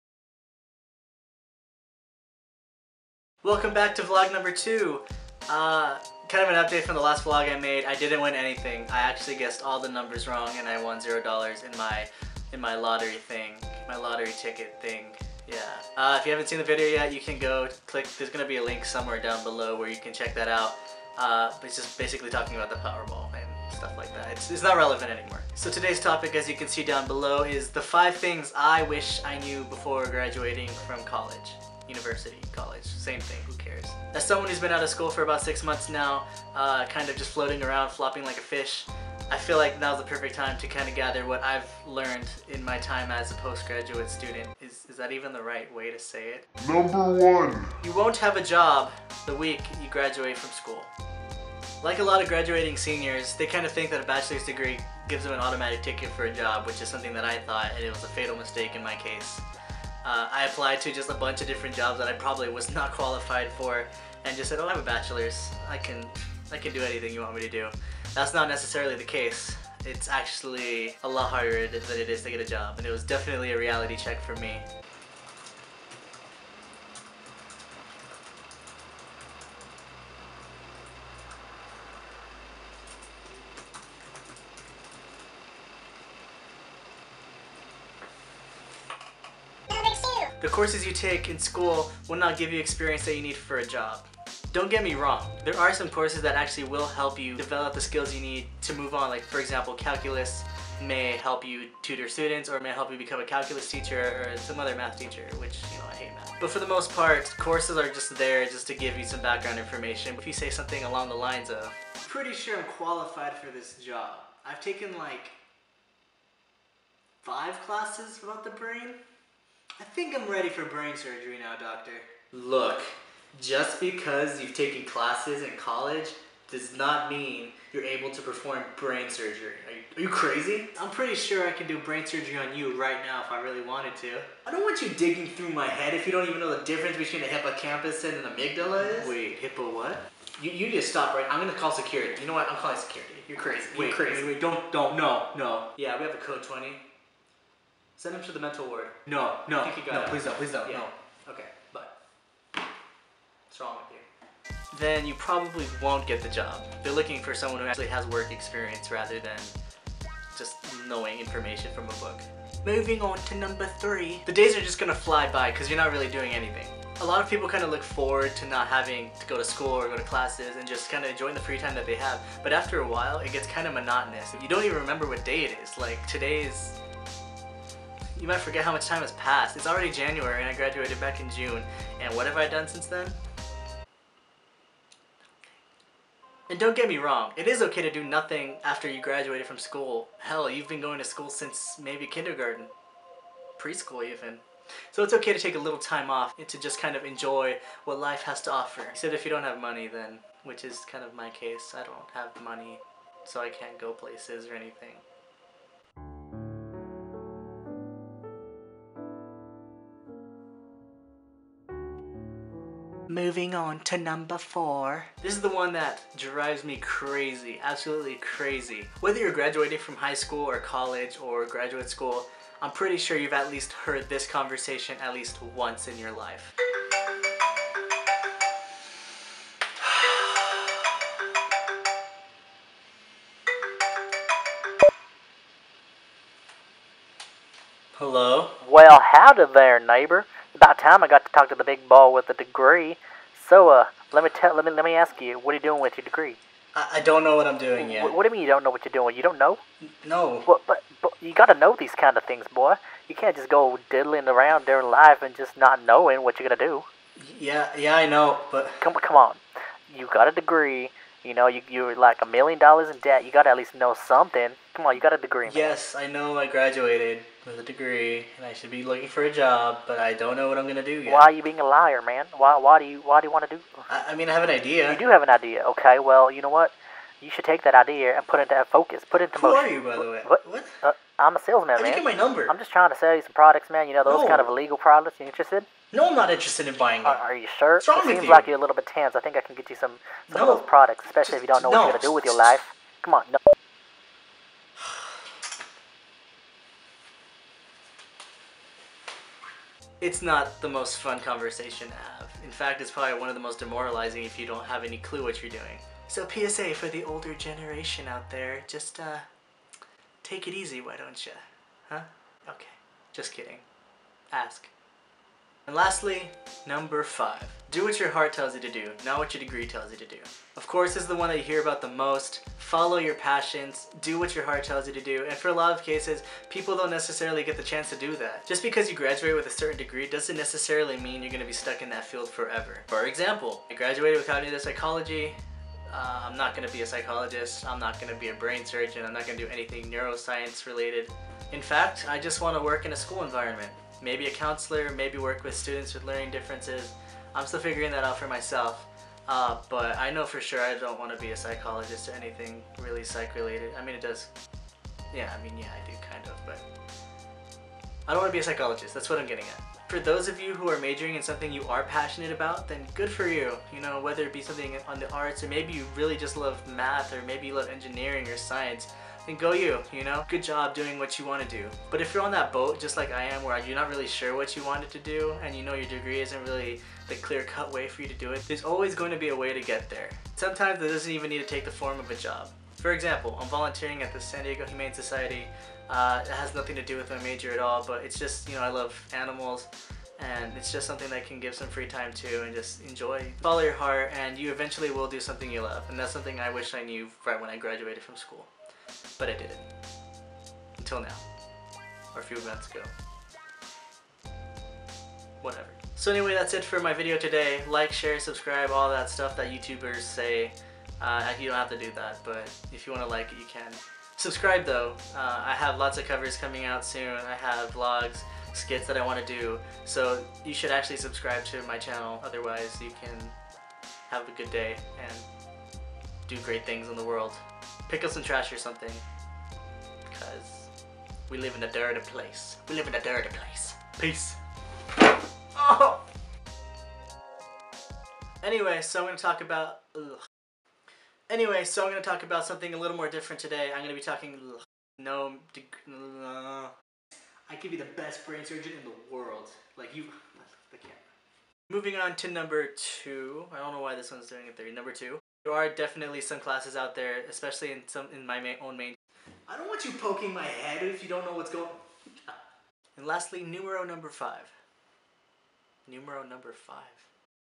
5 THINGS I WISH I KNEW BEFORE GRADUATING
Life is all about learning lessons. Here are 5 that I've learned after graduating.

*VIEWS ARE MY OWN AND NOT MEANT TO REPRESENT AN ENTIRE GROUP. (In before anyone gets all bootyhurt)

LIKE MY FACEBOOK PAGE FOR UPDATES, QUESTIONS, ETC.